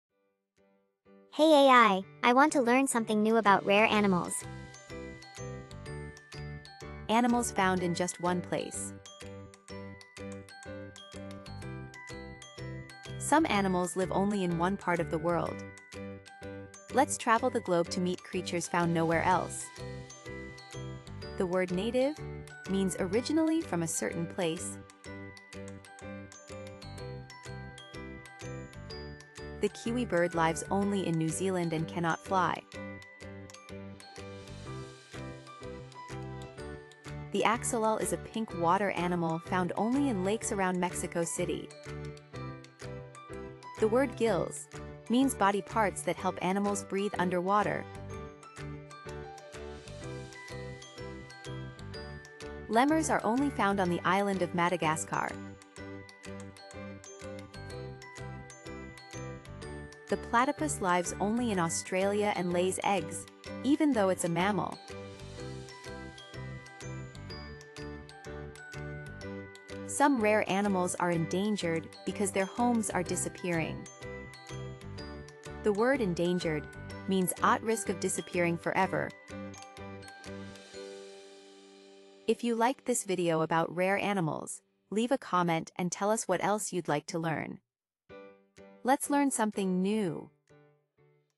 Animals Found in Just One Place
Learn something new about: "Rare Animals" : Some animals live only in one part of the world. Let’s travel the globe to meet creatures found nowhere else.
(Age 8–9)